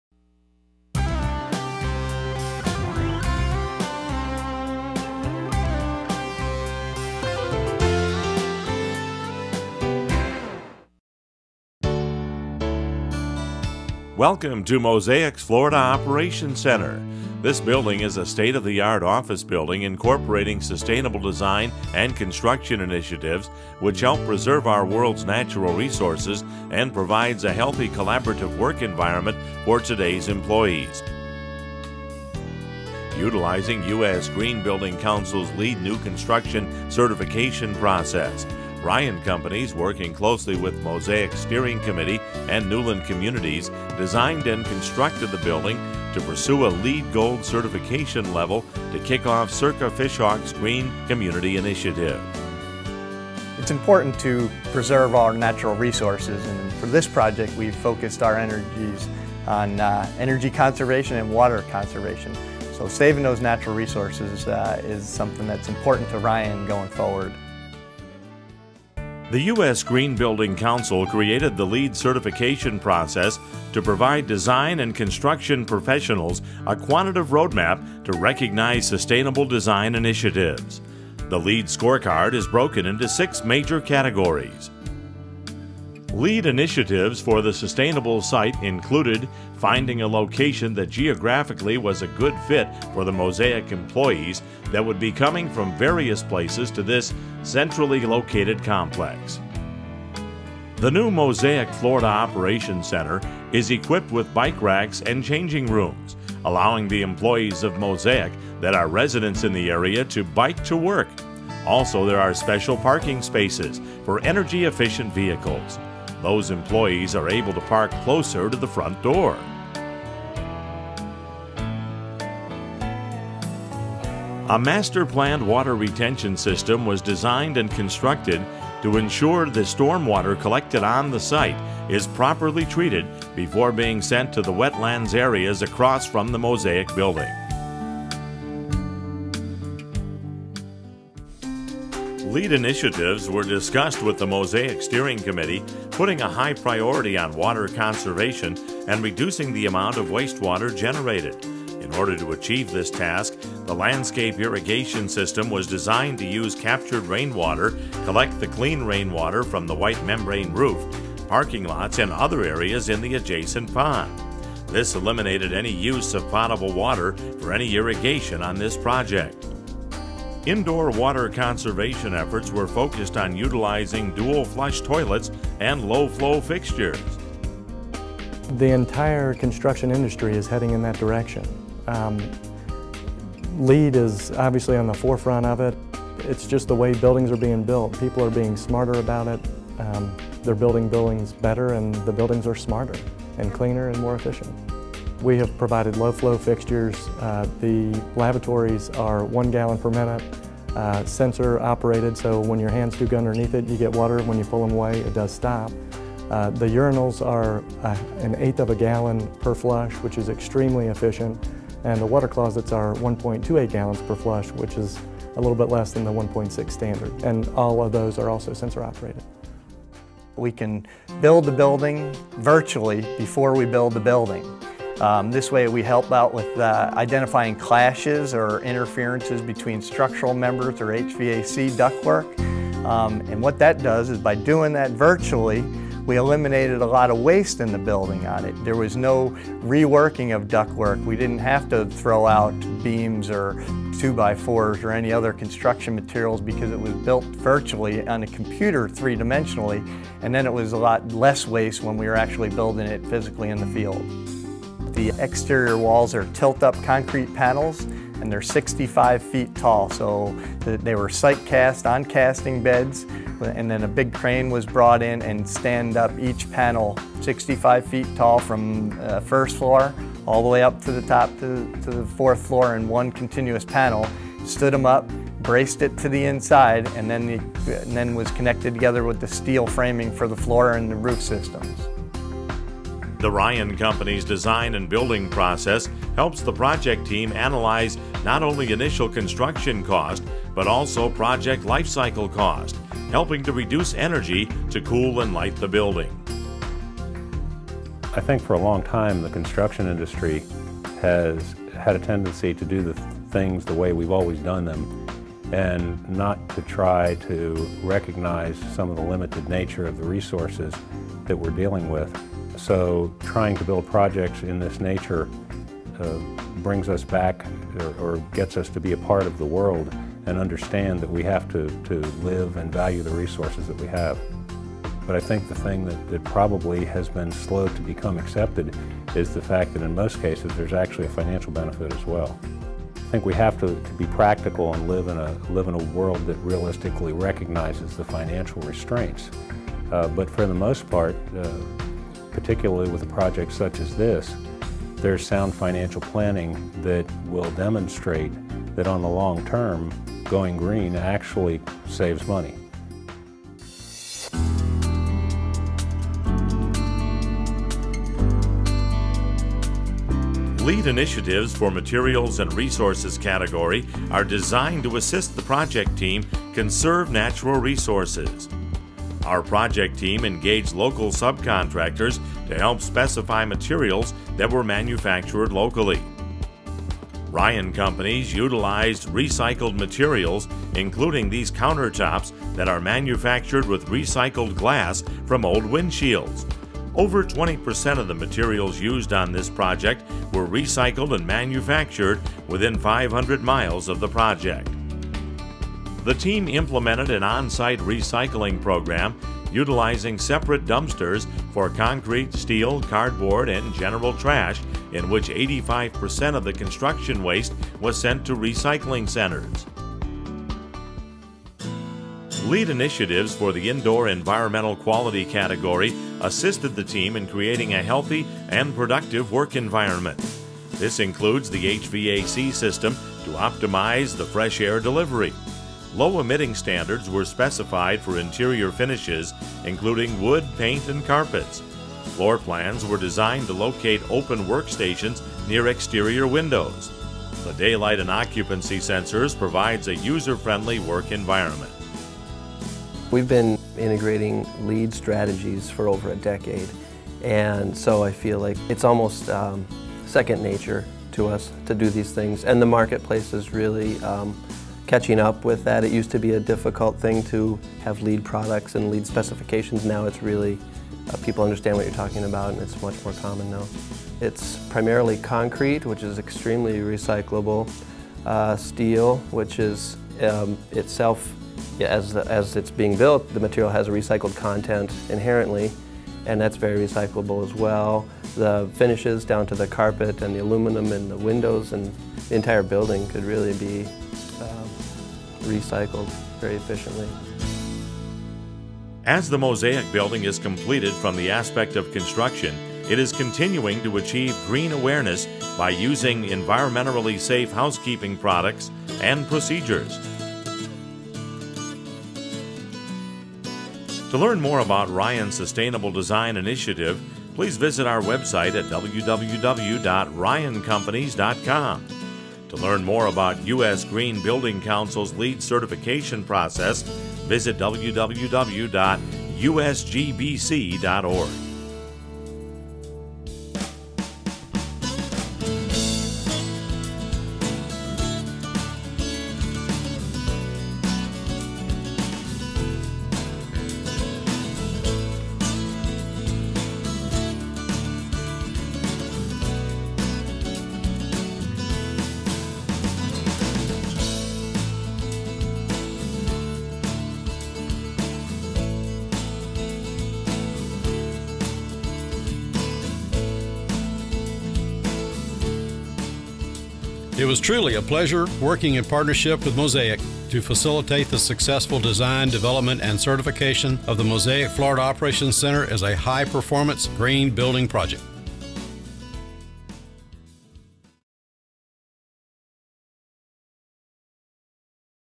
Ryan Companies LEED Video August 26th
Video Produced for Ryan Companies highlighting the building of the Mosaic Building in pursuing LEED certification